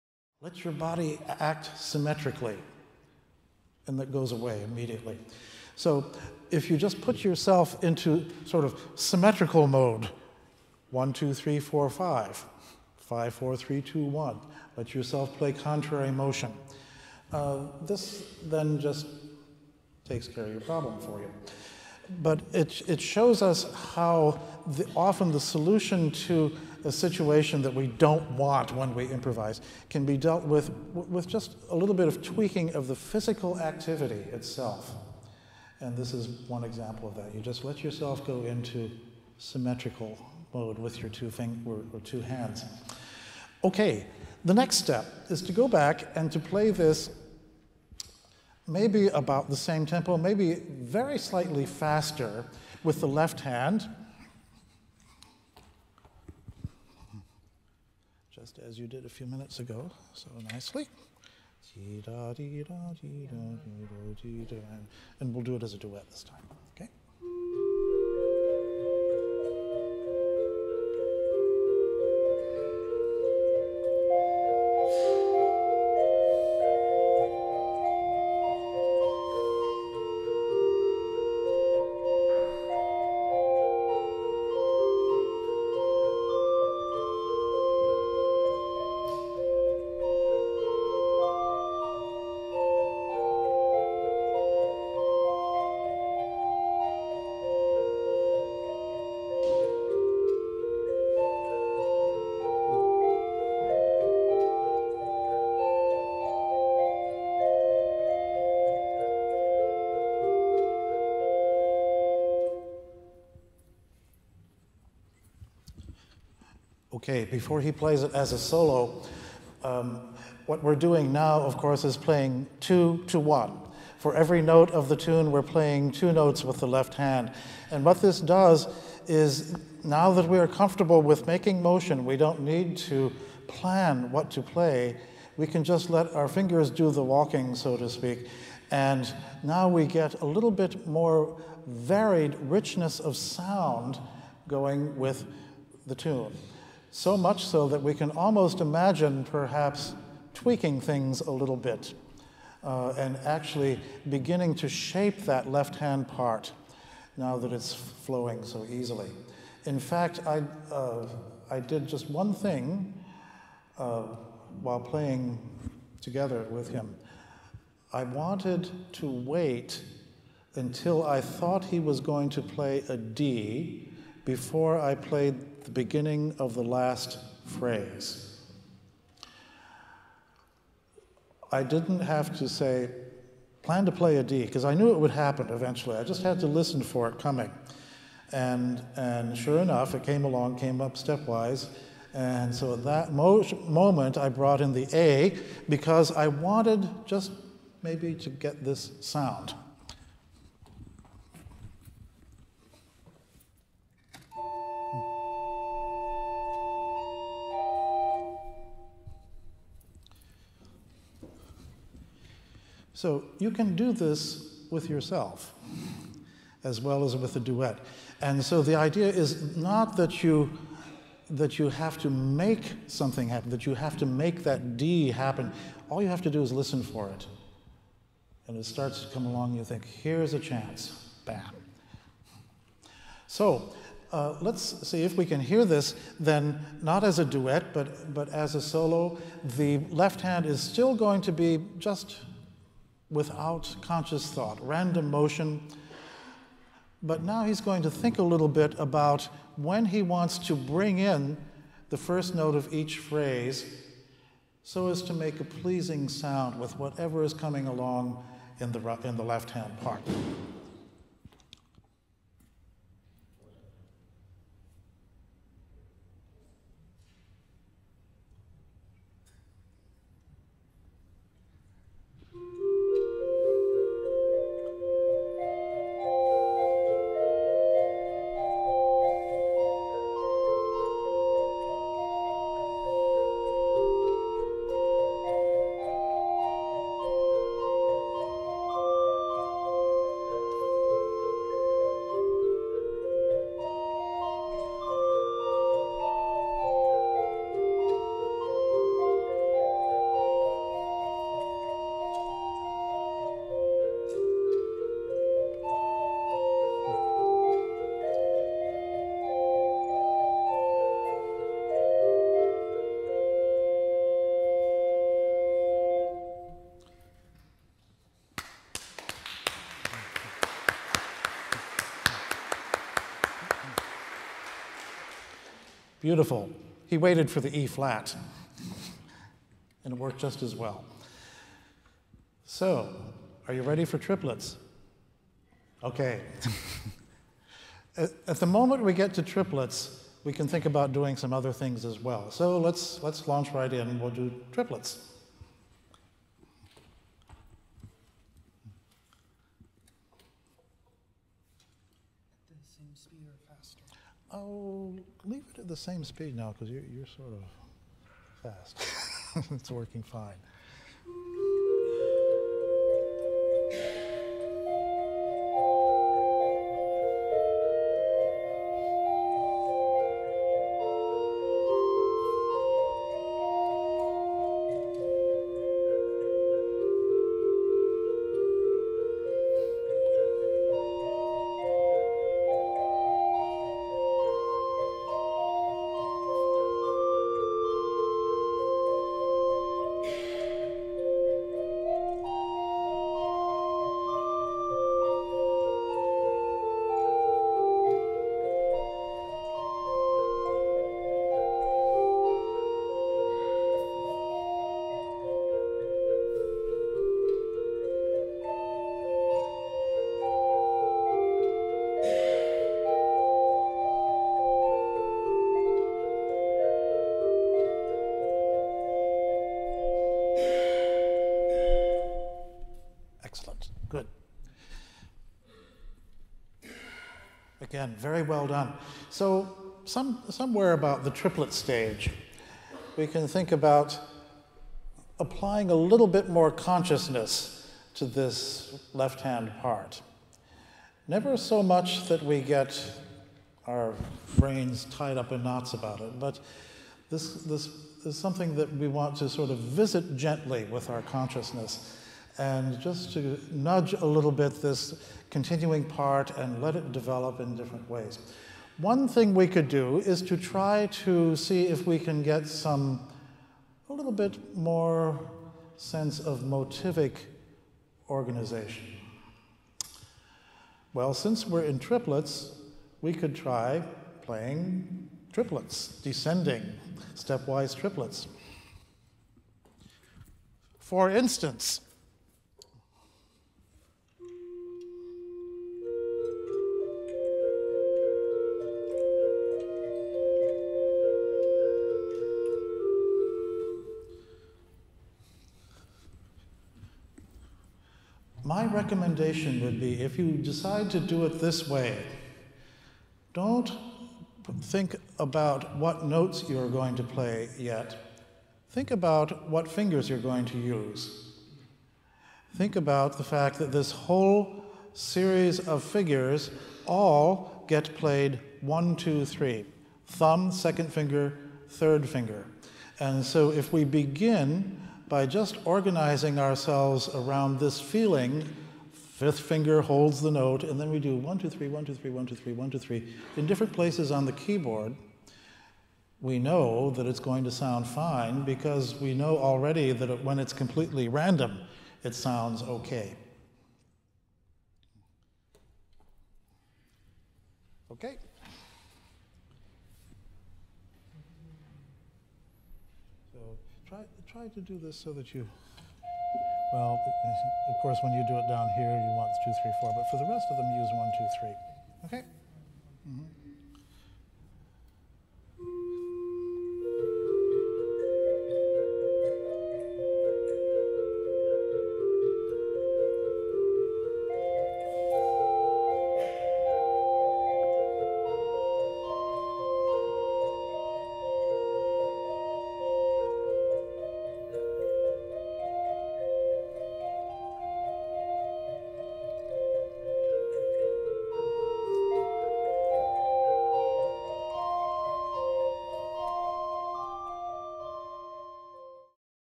Porter Workshop 2010 - Part 3
This video is the third of three video excerpts from workshop entitled "Hymn-Tune Improvisations: Increasing Your Skill," given by William Porter, as part of the 2010 National Convention of the American Guild of Organists (AGO) in Washington, D.C. Learn and explore the centuries' old heritage and history of the organ.  Improve your skill and enhance your career as an organist by joining the AGO today.